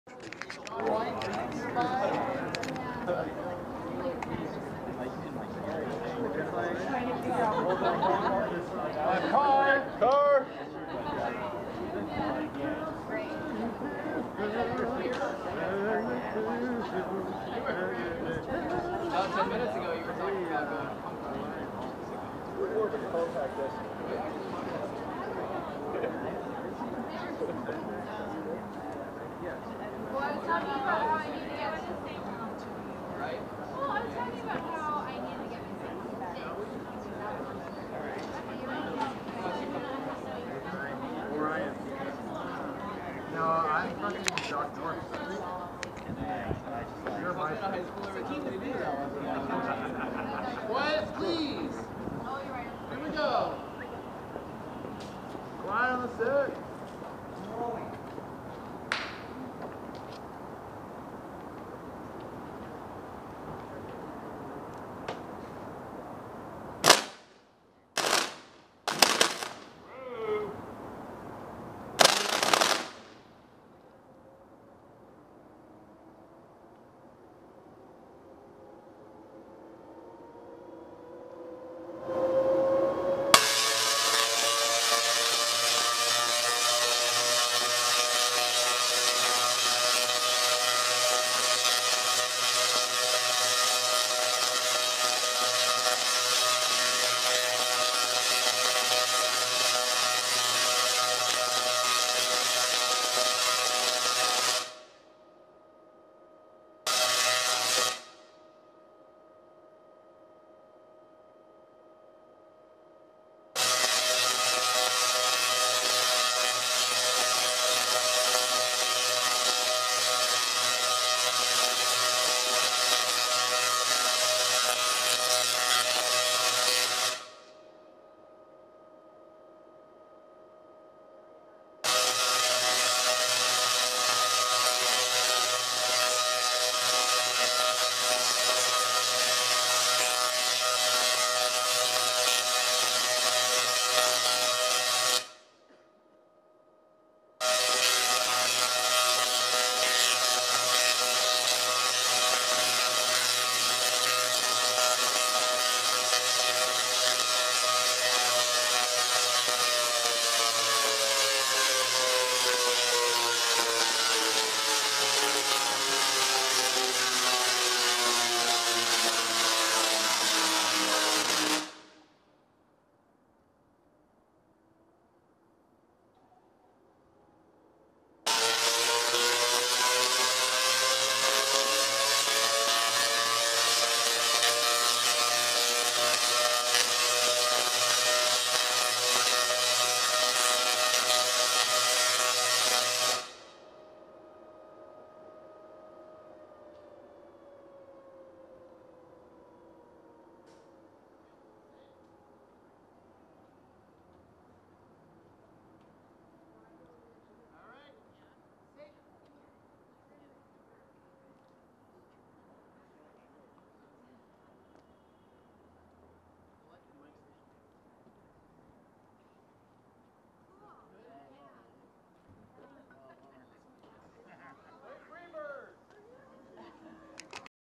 Omega Recoil: testing the Peg-R board with Tesla coil on Magnolia St.
Omega Recoil set up their big Tesla coil on Magnolia St in West Oakland and tested out a new effect concept.